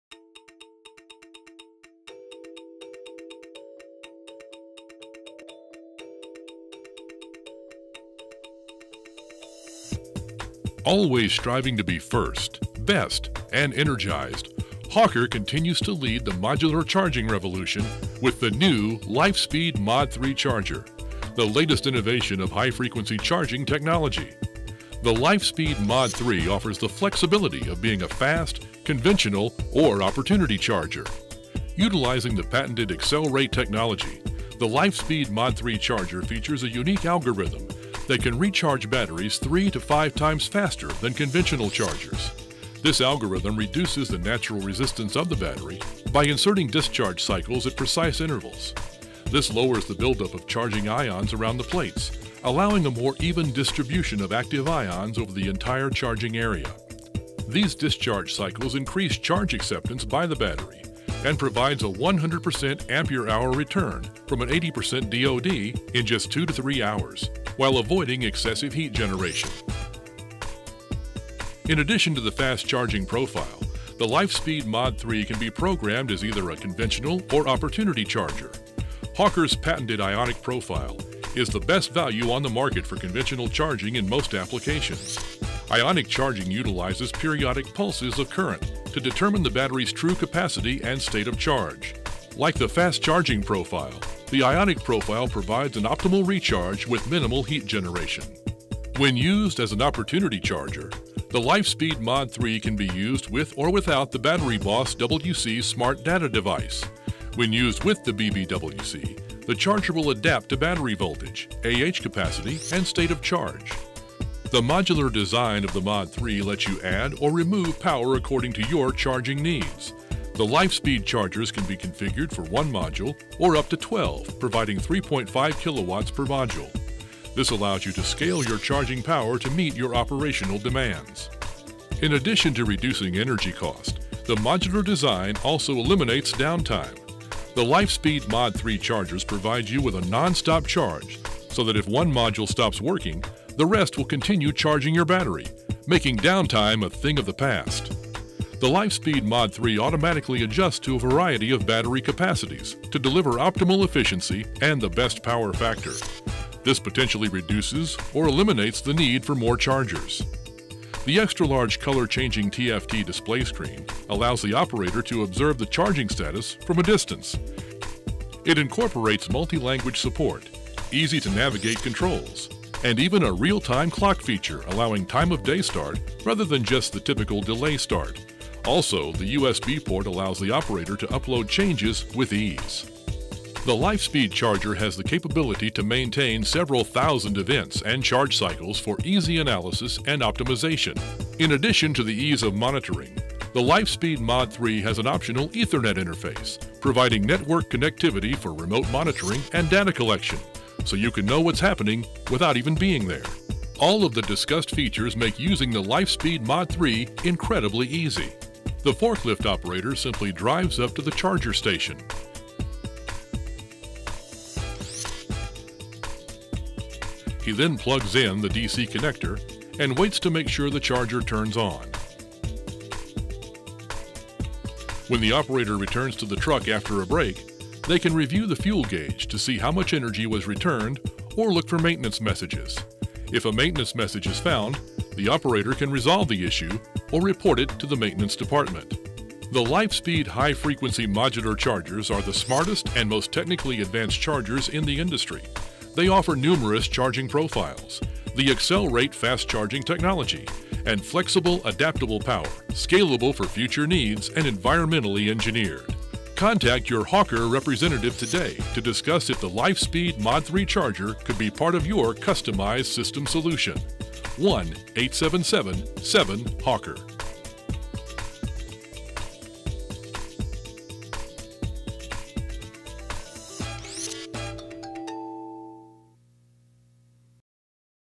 Introducing HAWKER LIFESPEED® MOD3 Modular Charger | Hawker Powersource, Inc.
The HAWKER® modular charging revolution continues with the new LIFESPEED® MOD3 charger - the latest innovation of high-frequency switch-mode smart chargers. Offering the flexibility of a fast charger, an opportunity charger, and a straight-through charger, the HAWKER LIFESPEED® MOD3 chargers provide optimum charging, peak efficiency, and non-stop power while reducing charging costs and energy demands.

-- More from Hawker Powersource, Inc. ---

--- Follow Us ---